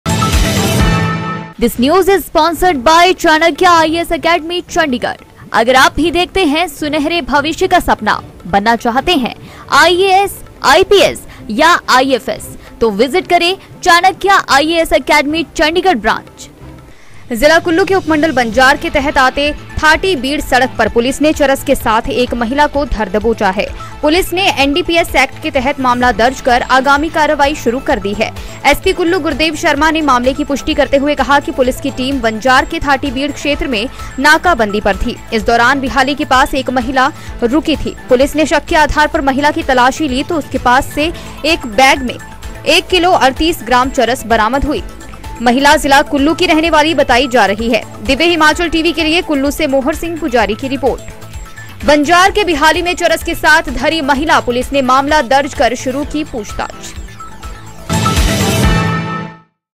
HimachalNews: बंजार के बिहाली में चरस के साथ धरी महिला, पुलिस ने मामला दर्ज कर शुरू की पूछताछ
divyahimachaltv
dhtv,tvnews
local news
divyahimachalgoodmorning
khabre ftafat
divyahimachalgoodafternoon